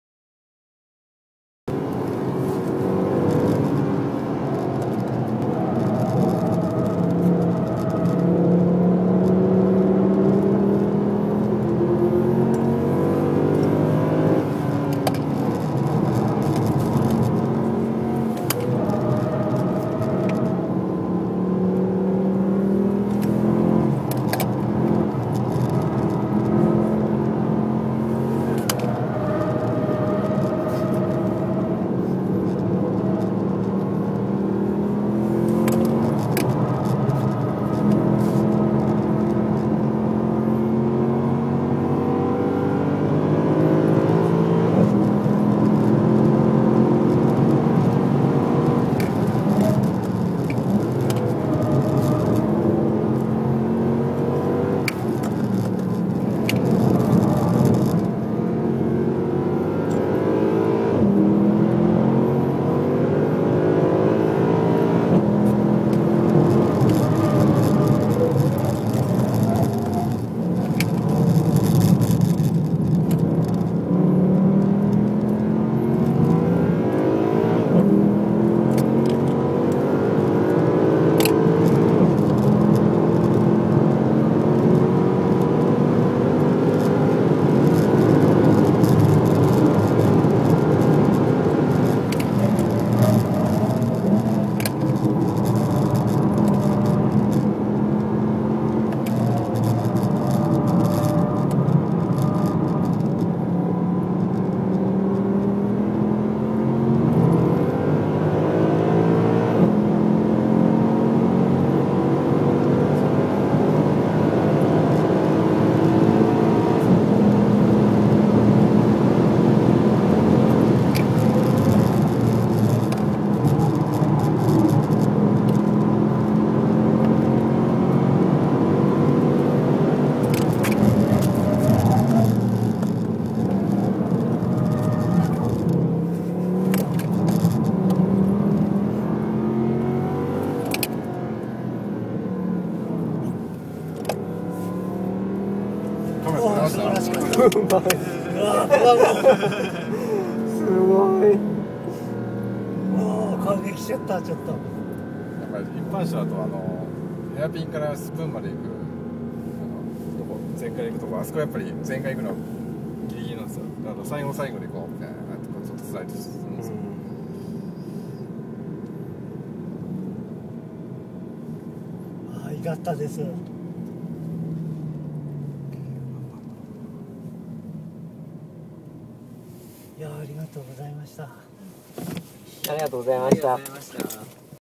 C63 AMG レーシングタクシー AMG Driving Fascination in Suzuka
AMG Driving Fascination in Suzuka Circuit
高木虎之介インストラクターがC63 AMGを運転。4名フル乗車。
先行するのは中谷彰彦総合インストラクターによるSLS AMG。